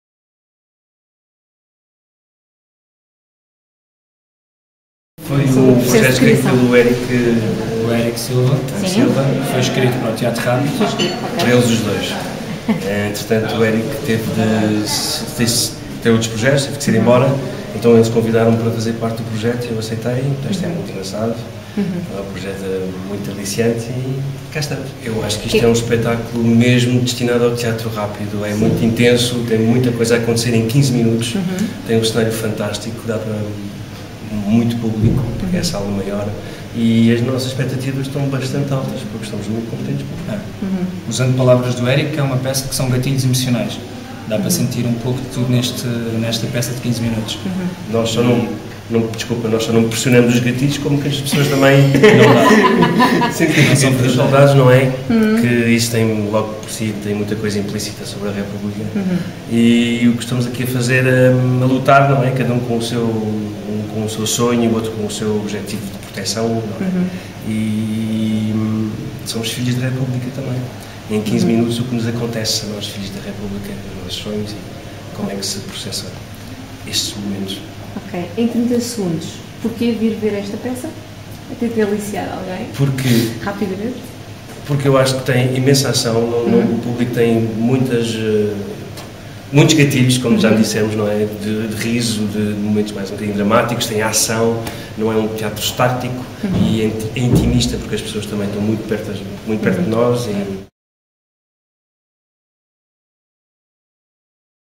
Hugo Costa Ramos e Philippe Leroux sobre "Último Brinde" @ Teatro Rapido
Com interpretação e encenação de Hugo Costa Ramos e Philippe Leroux, a peça baseia-se num texto de Eric L da Silva. Sala 1 do Teatro Rápido em Outubro 2012.